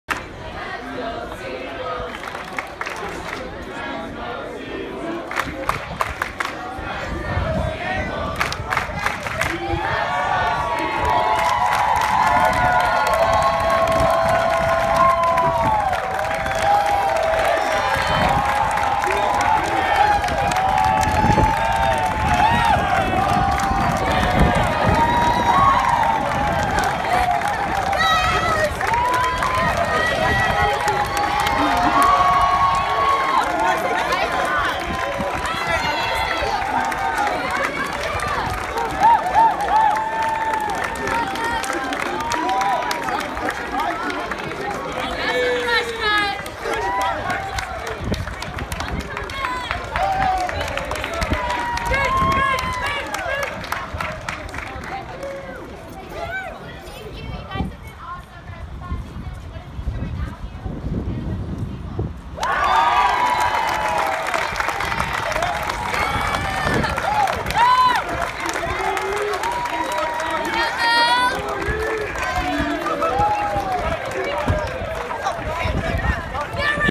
Seawolves Women's Volleyball Departs for NCAA Tournament
Fans wish Stony Brook's AE champions an enthusiastic farewell, Nov. 29, 2017.